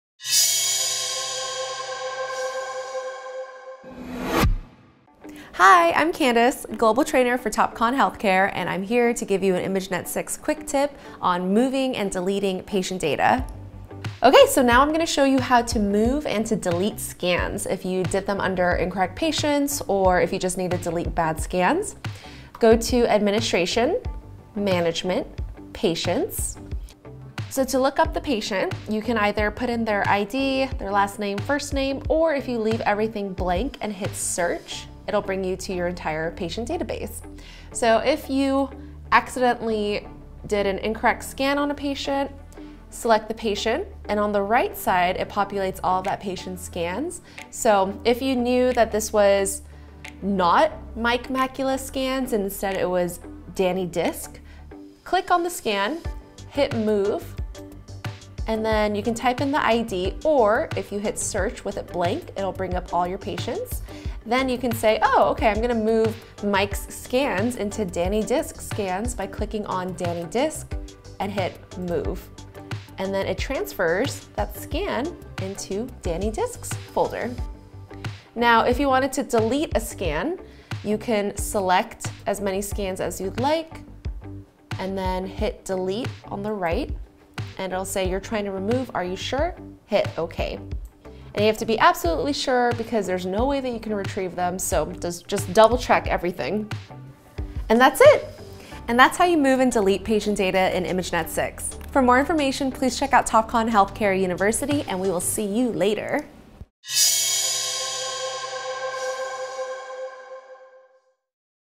IMAGEnet 6 Quick Tip - Move or Delete Patient Data
Managing patient data is essential to an efficient workflow. This brief quick tip video walks you through the process of moving and deleting patient data!